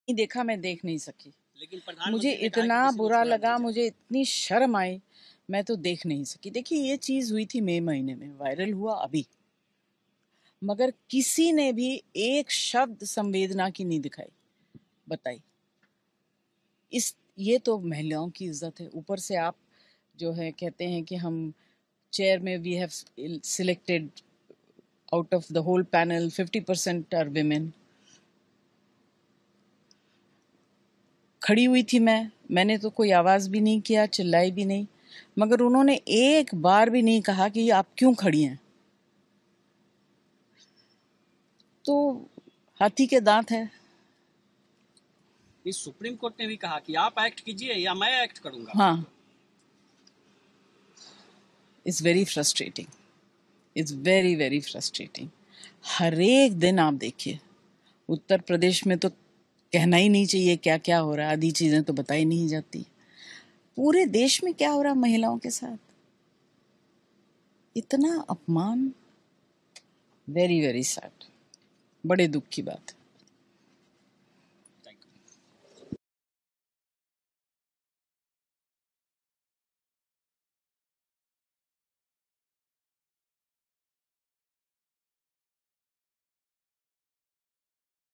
Jaya Bachchan: "शोक व्यक्त करण्यासाठी कोणी एक शब्दही...:; मणिपूरमधील घटनेवरून जया बच्चन यांचा संताप
Jaya Bachchan On Manipur: मणिपूरमध्ये जमावाने दोन महिलांची नग्न धिंड काढत त्यांच्यावर सामूहिक बलात्कार केल्याची संतापजनक घटना घडली आहे. घटनेचा व्हिडीओ सोशल मीडियावर व्हायरल झाल्यानंतर सर्वच स्तरातून संताप व्यक्त केला जात आहे. समाजवादी पक्षाच्या खासदार जया बच्चन यांनी देखील घटनेचा निषेध केला आहे. खरतंर ही घटना मे महिन्यात घडली होती. मात्र, त्याचा व्हिडीओ आता व्हायरल झाला असं त्या म्हणाल्या.